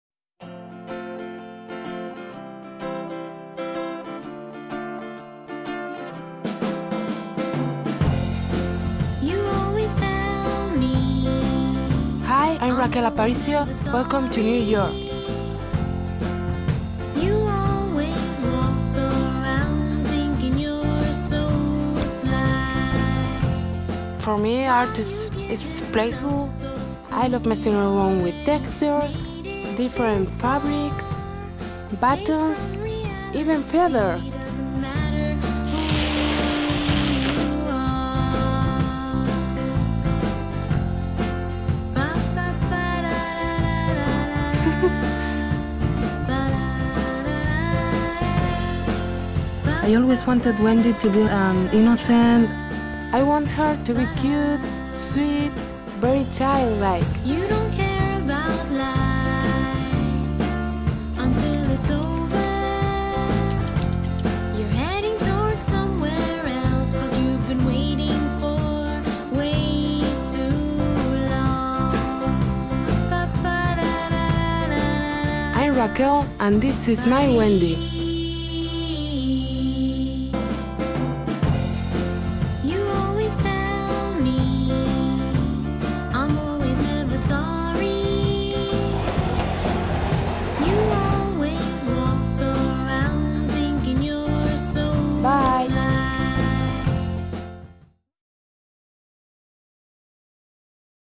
Wendy's - Create Your Wendy, Collage Artist
New York City collage artist creates her version of Wendy's logo from buttons, fabric & more.